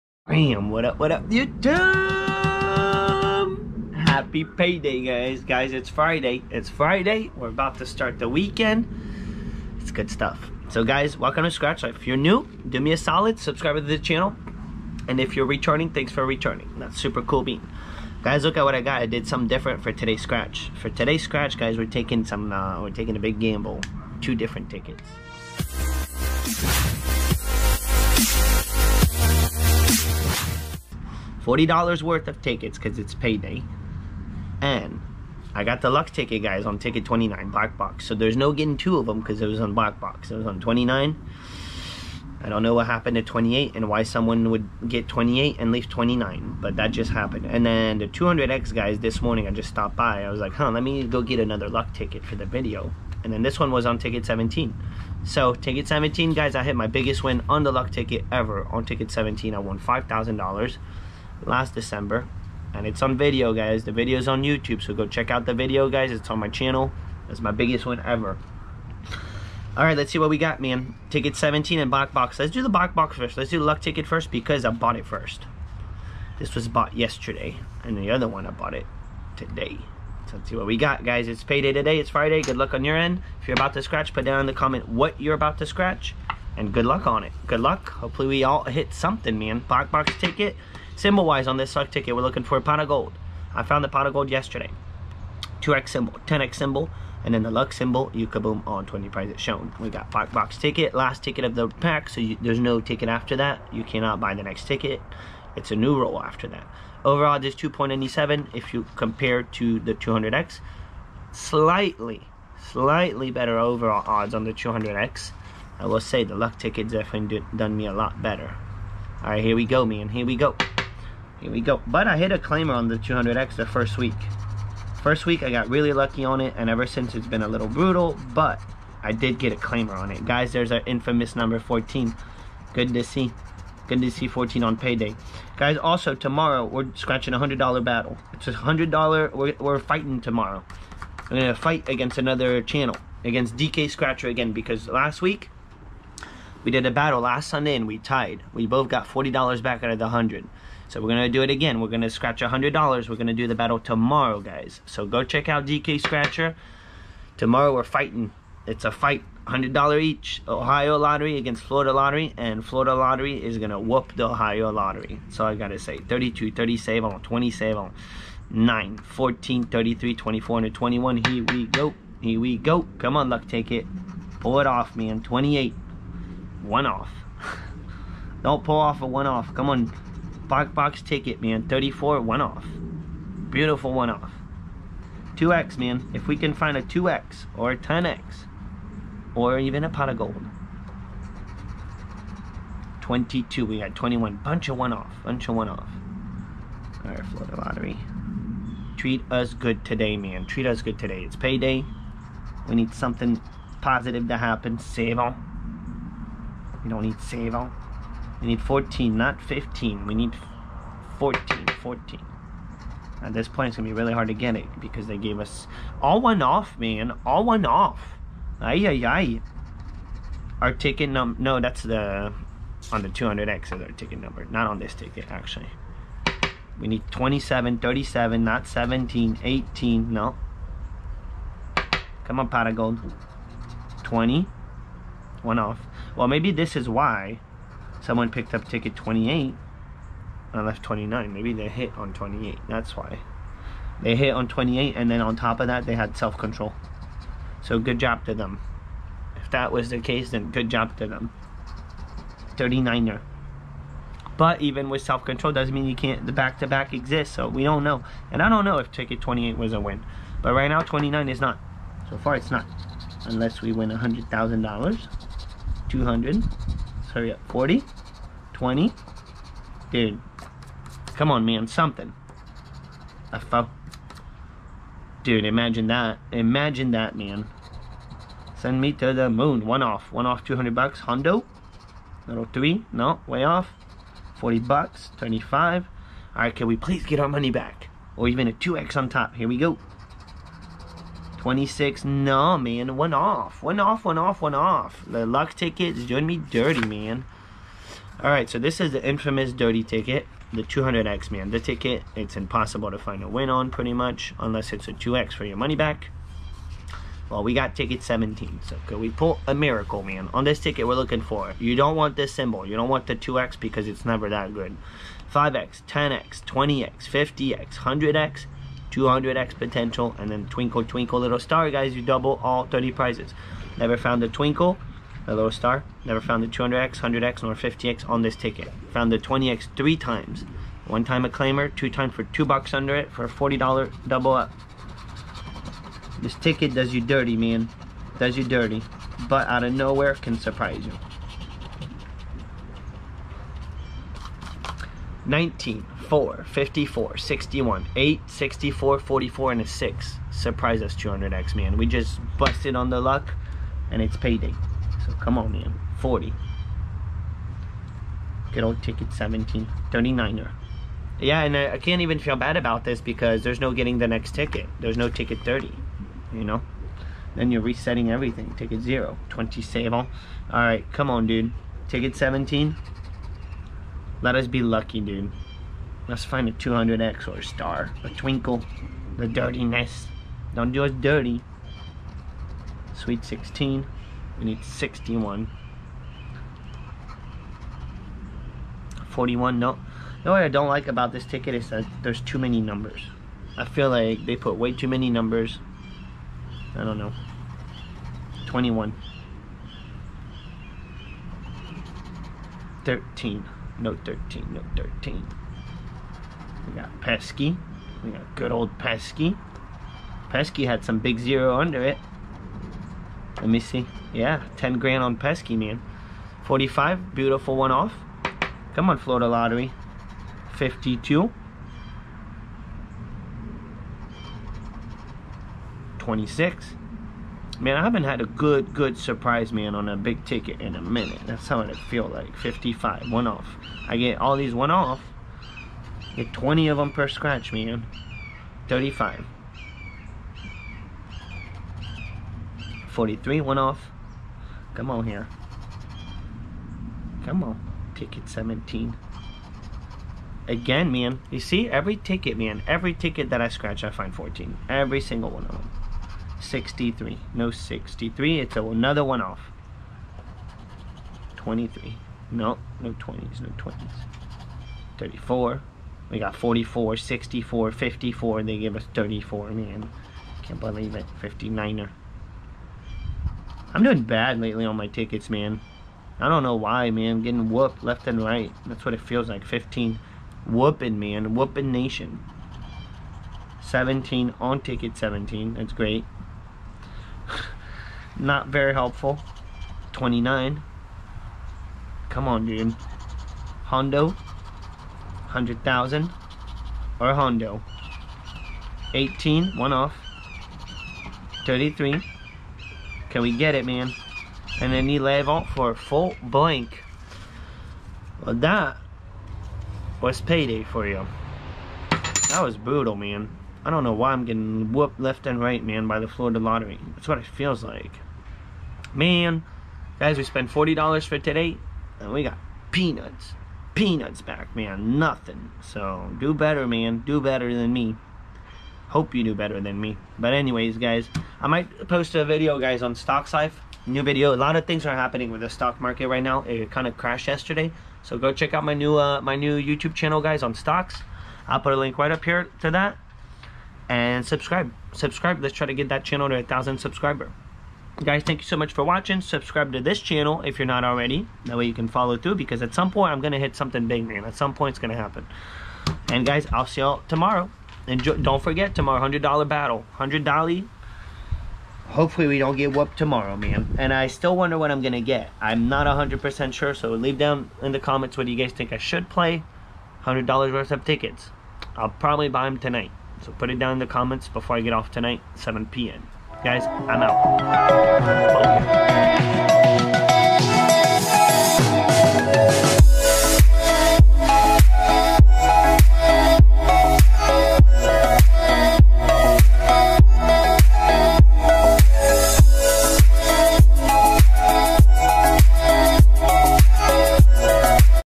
Bought Two, Its Payday! | $20 Scratch Off Tickets
Bought 2x $20 Scratch off tickets from the Florida Lottery!
$20 Luck Ticket
$20 200X the Cash Ticket